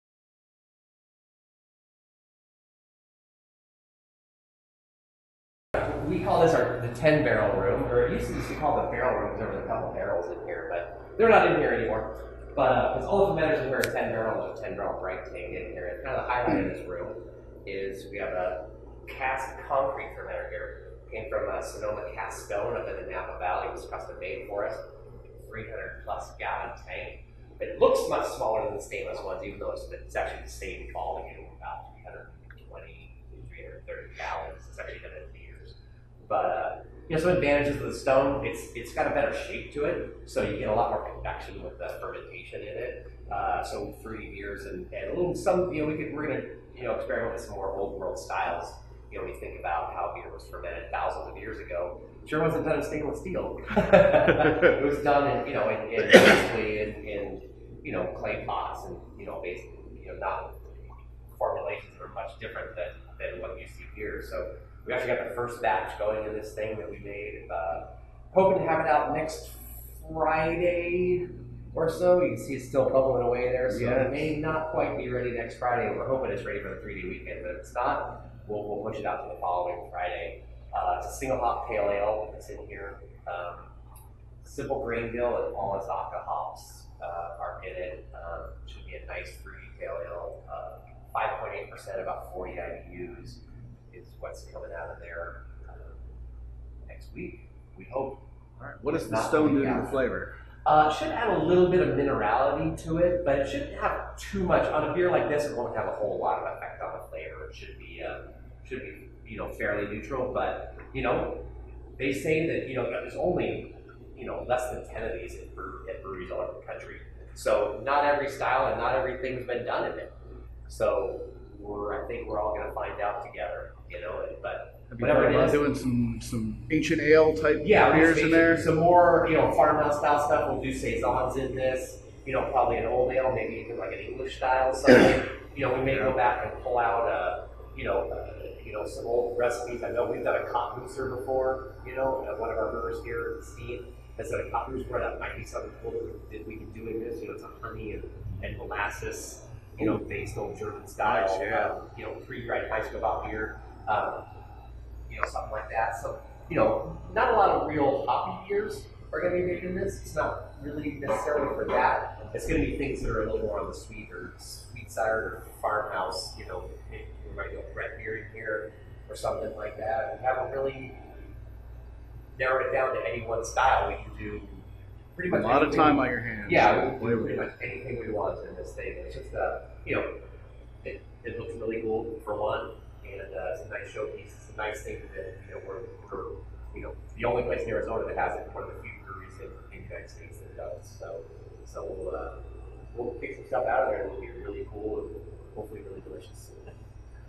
Perch Brewery Interview
I spoke with one of the brewers at Perch Brewery about their rare stone fermenter.